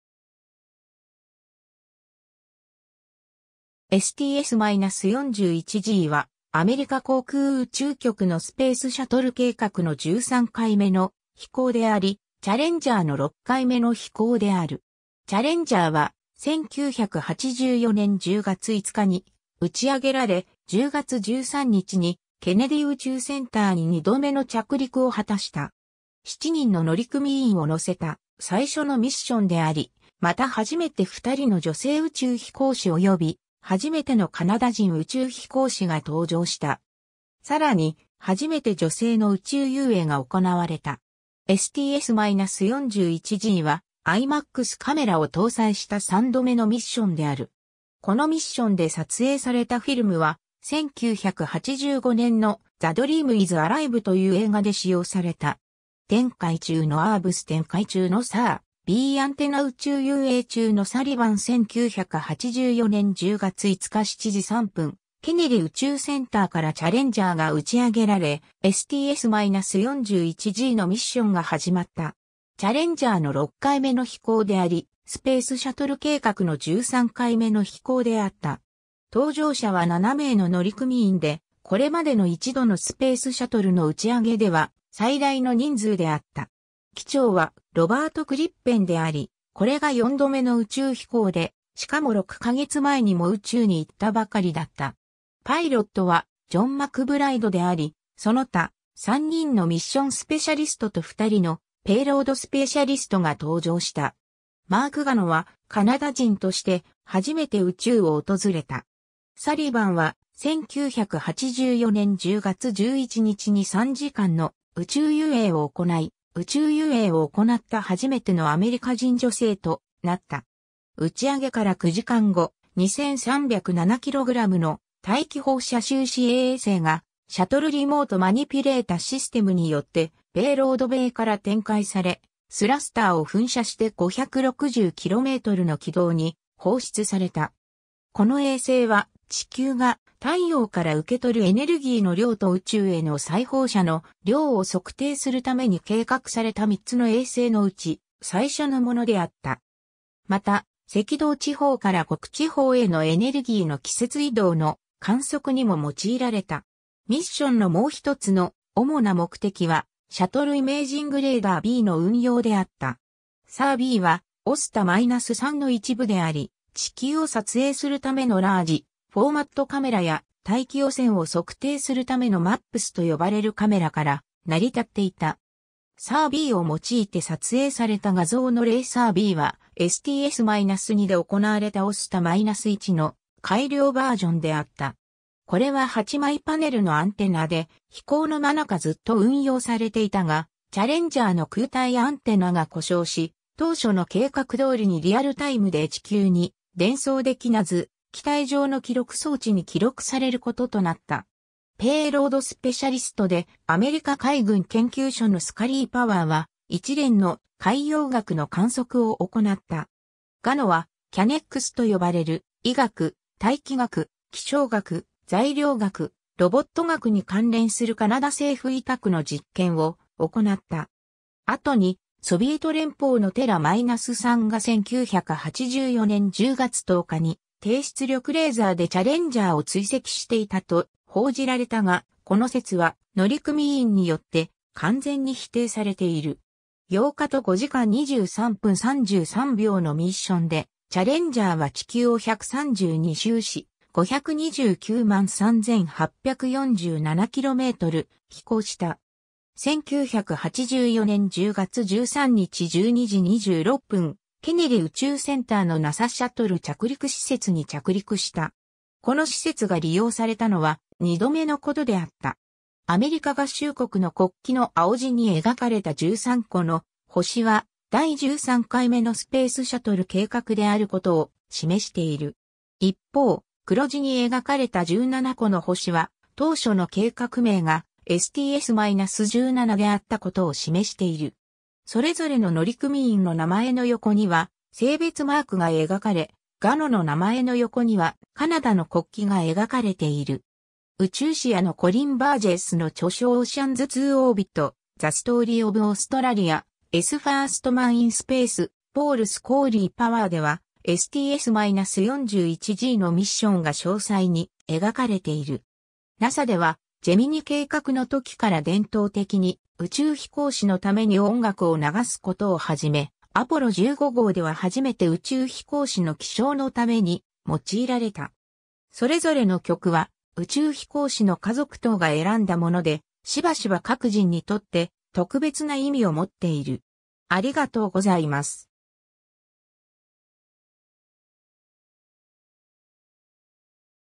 STS-41-G
STS-41-Gは、アメリカ航空宇宙局のスペースシャトル計画の13回目の飛行であり、チャレンジャーの6回目の飛行である。
チャレンジャーは、1984年10月5日に打ち上げられ、10月13日にケネディ宇宙センターに2度目の着陸を果たした。
7人の乗組員を乗せた最初のミッションであり、また初めて2人の女性宇宙飛行士（サリー・ライドとキャサリン・D・サリバン）及び、初めてのカナダ人宇宙飛行士（マーク・ガノ）が搭乗した。
さらに、初めて女性の宇宙遊泳（サリバン）が行われた。
 STS-41-Gは、IMAXカメラを搭載した3度目のミッションである。
このミッションで撮影されたフィルム（サリバンとリーストマの宇宙遊泳を含む）は、1985年のThe Dream Is Aliveという映画で使用された。
 展開中のERBS 展開中のSIR-Bアンテナ 宇宙遊泳中のサリバン 1984年10月5日7時3分(EDT)、ケネディ宇宙センターからチャレンジャーが打ち上げられ、STS-41-Gのミッションが始まった。
チャレンジャーの6回目の飛行であり、スペースシャトル計画の13回目の飛行であった。
 搭乗者は7名の乗組員で、これまでの1度のスペースシャトルの打上げでは最大の人数であった。
機長はロバート・クリッペンであり、これが4度目の宇宙飛行で、しかも6か月前にも宇宙に行ったばかりだった。
パイロットはジョン・マクブライドであり、その他、3人のミッションスペシャリスト（デビッド・リーストマ、サリー・ライド、キャサリン・D・サリバン）と2人のペイロードスペシャリスト（ポール・スカリー=パワーとマーク・ガノ）が搭乗した。
マーク・ガノは、カナダ人として初めて宇宙を訪れた。
 サリバンは、1984年10月11日に3時間の宇宙遊泳を行い、宇宙遊泳を行った初めてのアメリカ人女性となった。
 打上げから9時間後、2,307kgの大気放射収支衛星（ERBS）がシャトル・リモート・マニピュレータ・システムによってペイロードベイから展開され、スラスターを噴射して560㎞の軌道に放出された。
この衛星は、地球が太陽から受け取るエネルギーの量と宇宙への再放射の量を測定するために計画された3つの衛星のうち最初のものであった。
また、赤道地方から極地方へのエネルギーの季節移動の観測にも用いられた。
 ミッションのもう1つの主な目的は、Shuttle Imaging Radar-B (SIR-B)の運用であった。
SIR-BはOSTA-3の一部であり、地球を撮影するためのLarge Format Camera (LFC)や大気汚染を測定するためのMAPSと呼ばれるカメラから成り立っていた。
 SIR-Bを用いて撮影された画像の例 SIR-Bは、STS-2で行われたOSTA-1の改良バージョンであった。
これは8枚パネルのアンテナで、飛行の間中ずっと運用されていたが、チャレンジャーのKu帯アンテナが故障し、当初の計画通りにリアルタイムで地球に伝送できなず、機体上の記録装置に記録されることとなった。
 ペイロードスペシャリストでアメリカ海軍研究所のスカリー=パワーは、一連の海洋学の観測を行った。
ガノは、CANEXと呼ばれる、医学、大気学、気象学、材料学、ロボット学に関連するカナダ政府委託の実験を行った。
 後に、ソビエト連邦のTerra-3が1984年10月10日に低出力レーザーでチャレンジャーを追跡していたと報じられたが、この説は乗組員によって完全に否定されている。
 8日と5時間23分33秒のミッションで、チャレンジャーは地球を132周し、5,293,847km飛行した。
1984年10月13日12時26分(EDT)、ケネディ宇宙センターのNASAシャトル着陸施設に着陸した。
この施設が利用されたのは、2度目のことであった。
 アメリカ合衆国の国旗の青地に描かれた13個の星は、第13回目のスペースシャトル計画であることを示している。
一方、黒地に描かれた17個の星は、当初の計画名がSTS-17であったことを示している。
それぞれの乗組員の名前の横には性別マークが描かれ、ガノの名前の横にはカナダの国旗が描かれている。
 宇宙史家のコリン・バージェスの著書Oceans to Orbit: The Story of Australia's First Man in Space, Paul Scully-Powerで...